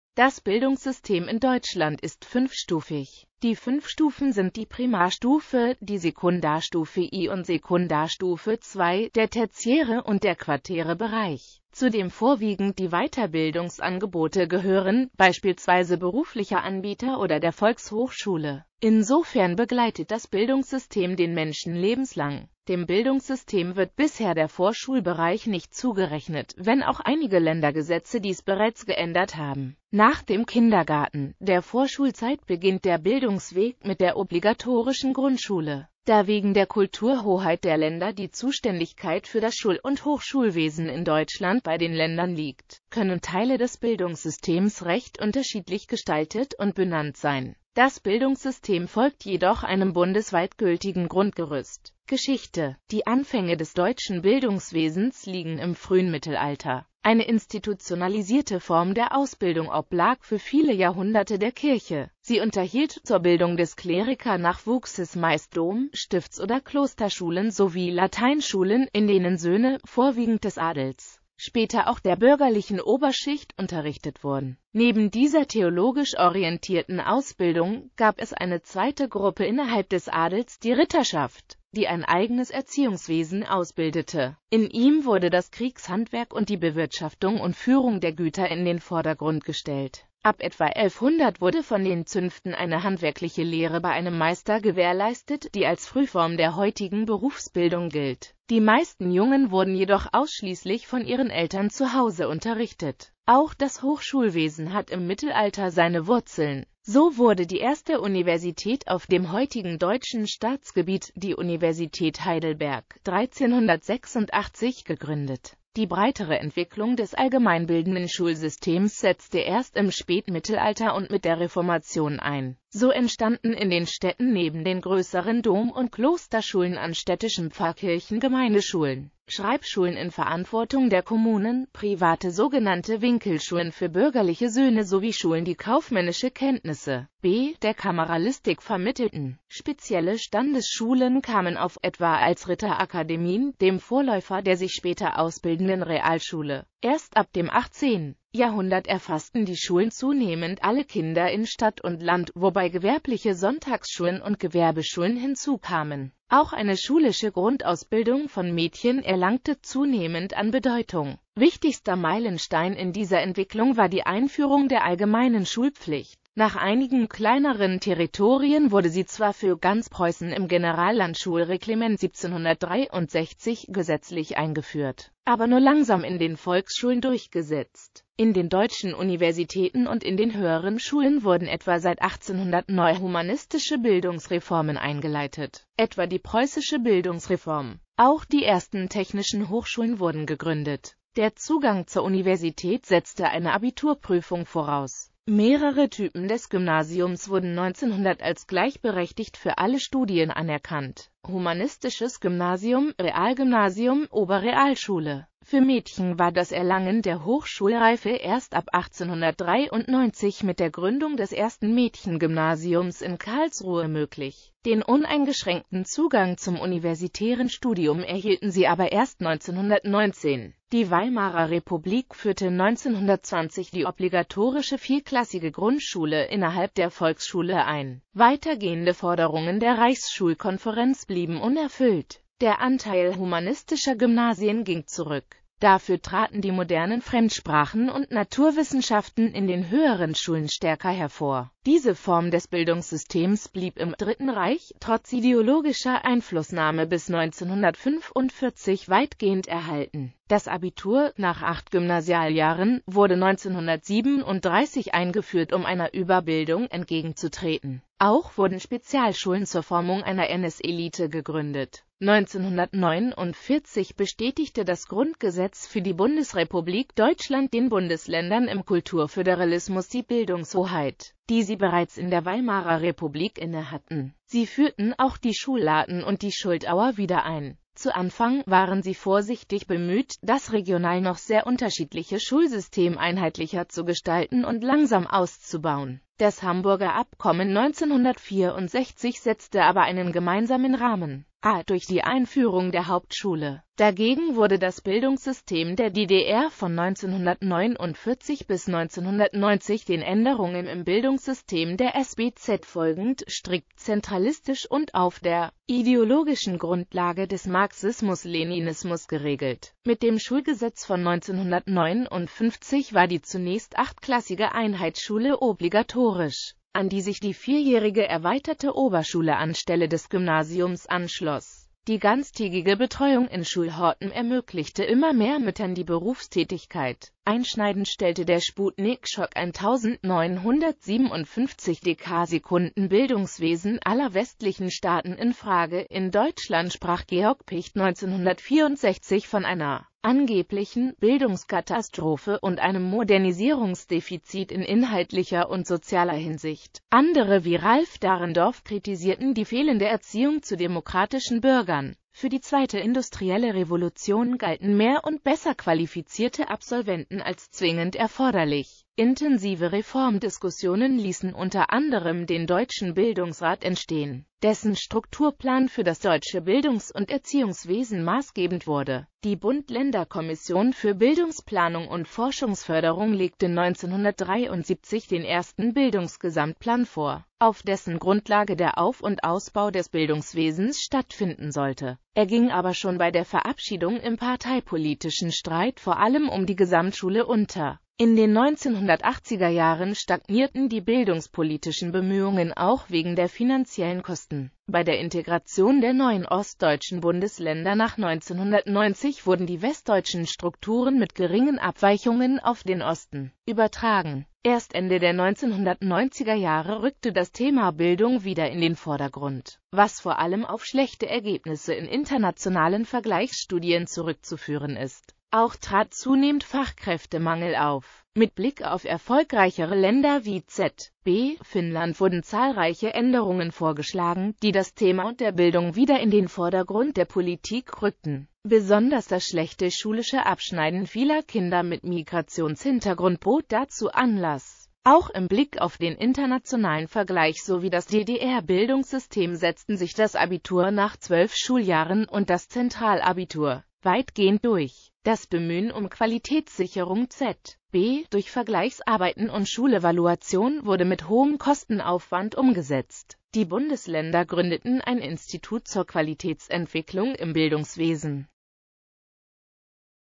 Bildungssystem in Deutschland
Vielen Dank für Ihre Unterstützung:

Das Bildungssystem in Deutschland ist fünfstufig.Die fünf Stufen sind die Primarstufe, die Sekundarstufe I und Sekundarstufe II, der tertiäre und der quartäre Bereich, zu dem vorwiegend die Weiterbildungsangebote gehören, beispielsweise beruflicher Anbieter oder der Volkshochschule.Insofern begleitet das Bildungssystem den Menschen lebenslang.Dem Bildungssystem wird bisher der Vorschulbereich nicht zugerechnet, wenn auch einige Ländergesetze dies bereits geändert haben.

Bild-Copyright-Informationen
Urheber Info: maix¿?
Lizenz: Creative Commons ShareAlike 1.0 Generic (CC SA 1.0)
Urheber: